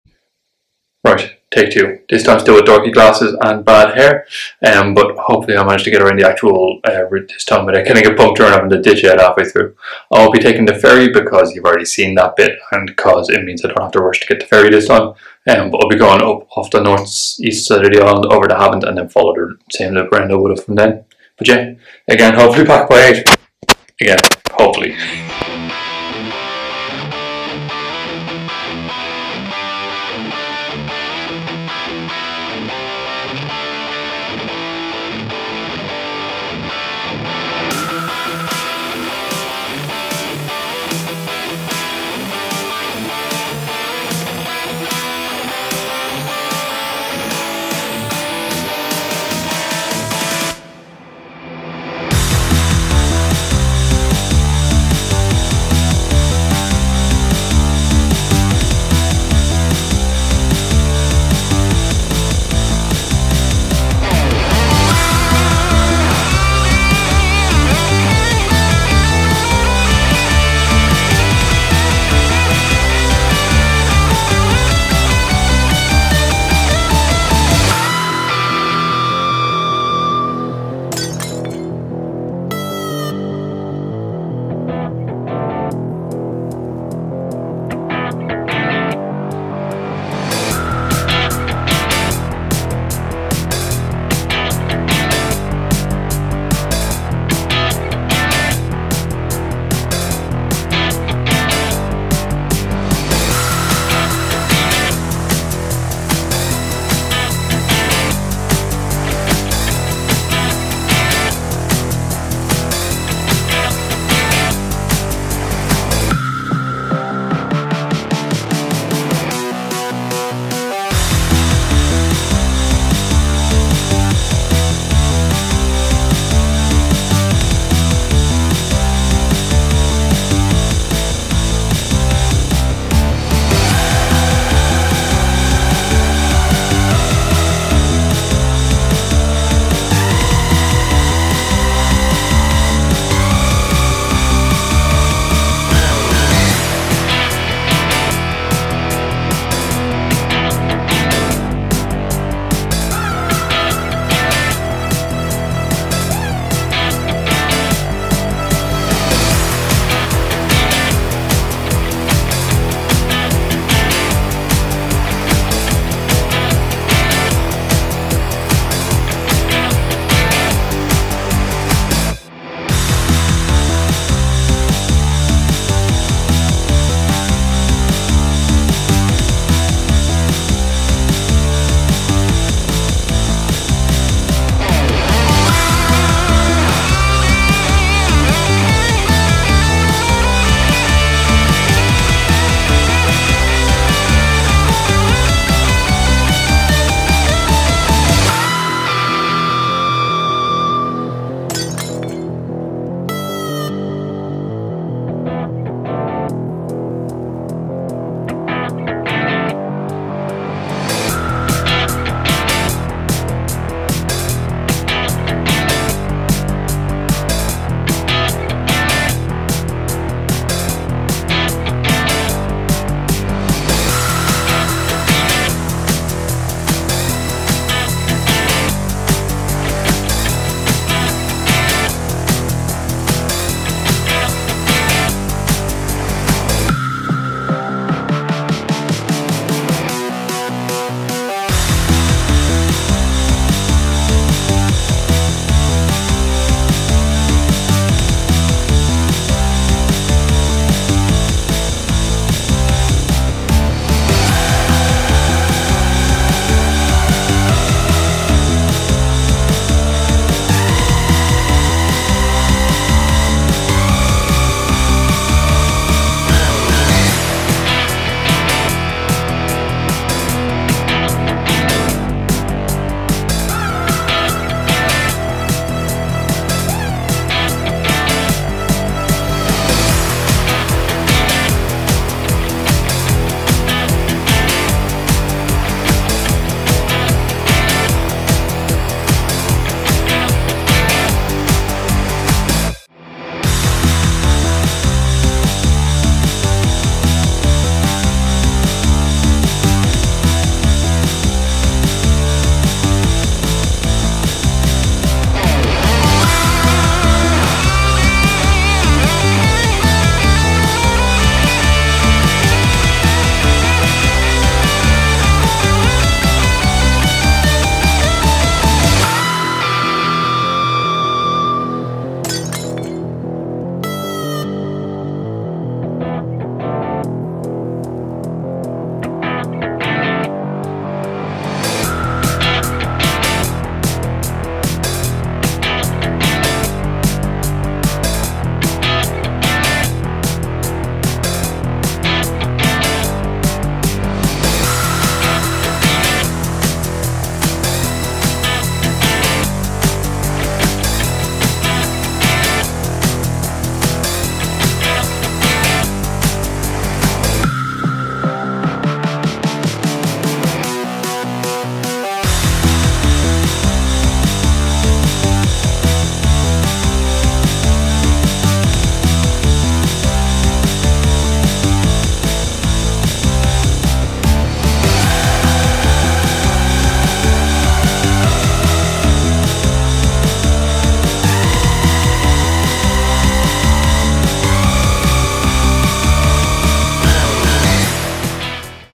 Emsworth Loop Vengeance
Having Punctured yesterday and had to bail out of the rest of the route, today was some what soggier but more successful.